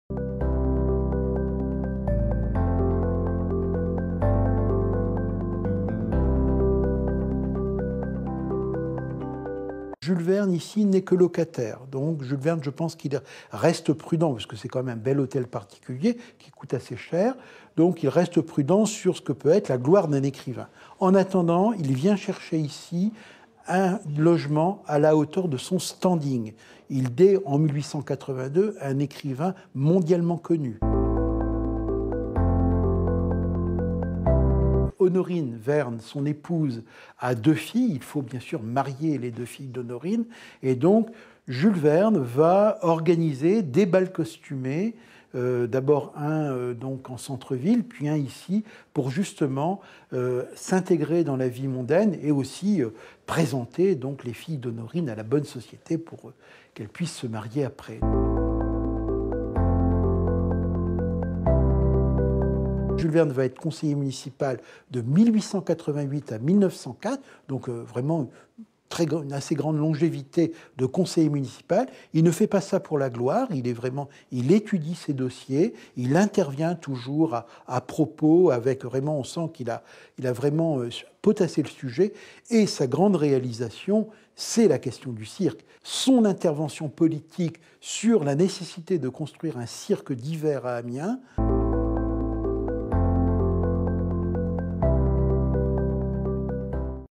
Trois questions à Bernard Sinoquet, responsable de la Maison de Jules Verne
Bernard Sinoquet raconte la vie menée par Jules Verne à Amiens, où il a vécu pendant 34 ans. Entre vie de famille, vie culturelle et vie politique, l'écrivain nantais a marqué la ville de Picardie.

Reportage de : Eléonore Duplay, Jean-Paul Delance, Jérôme Arrignon et Philippe Coat.